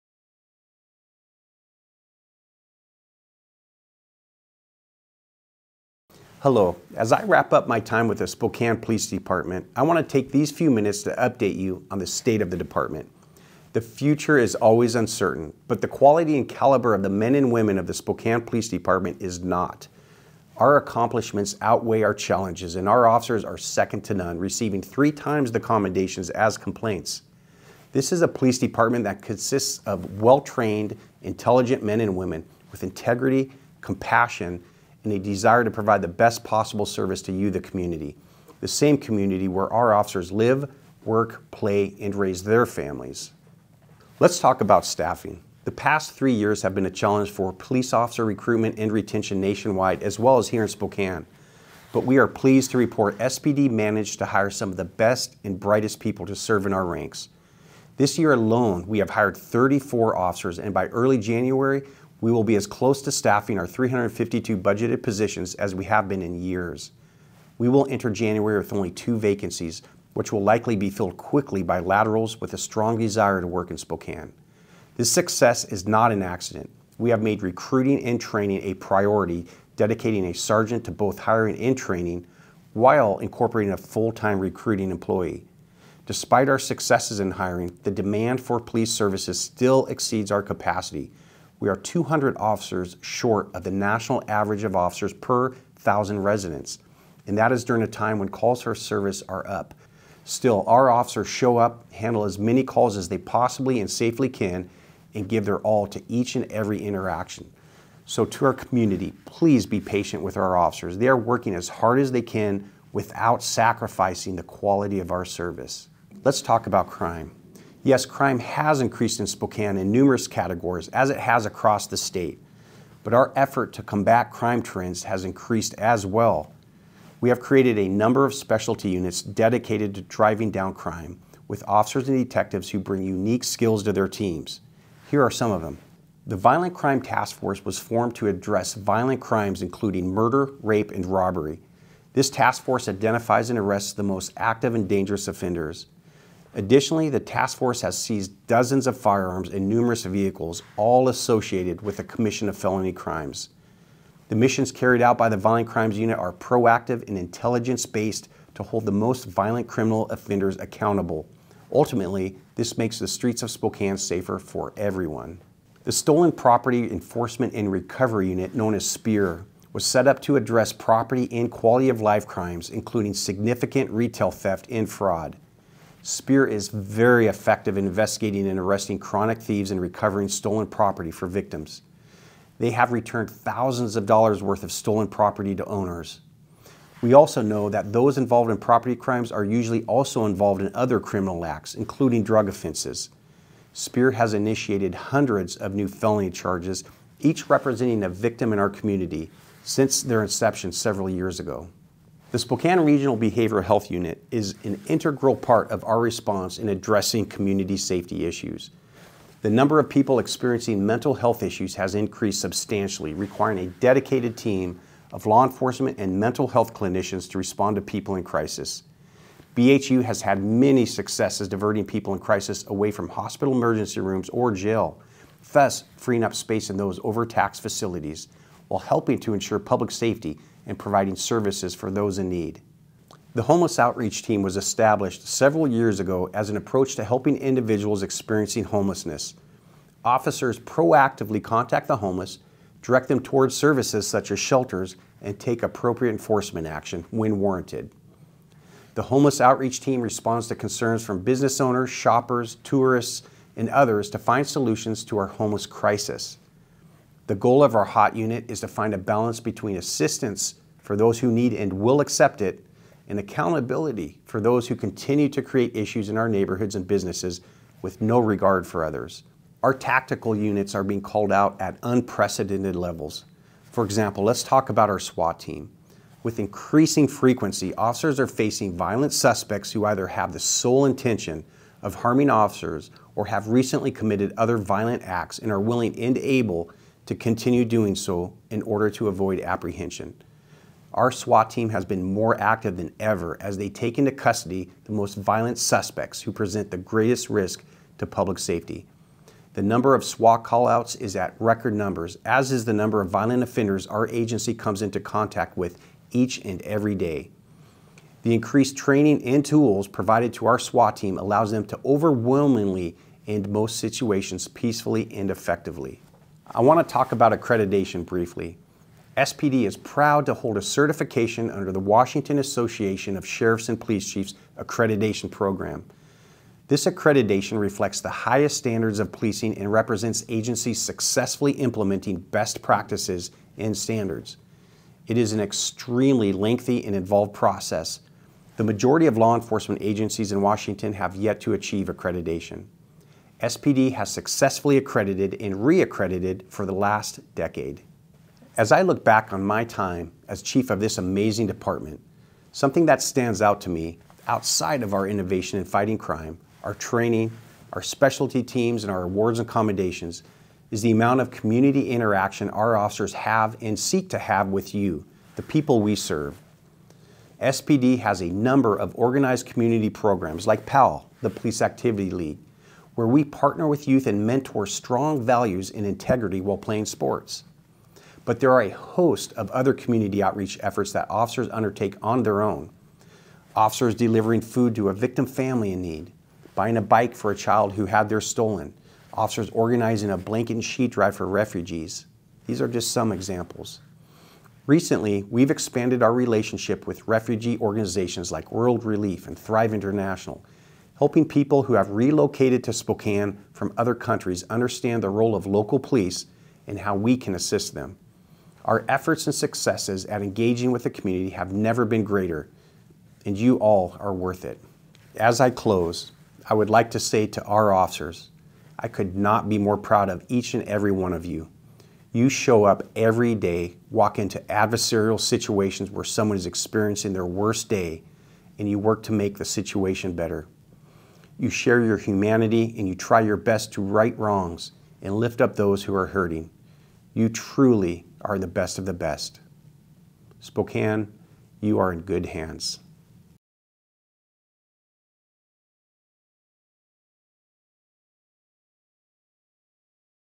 Meidl's Minute - State of the Department
Chief Meidl looks back at his time with SPD and updates you on the state of the police department. Please enjoy this final Meidl’s Minute.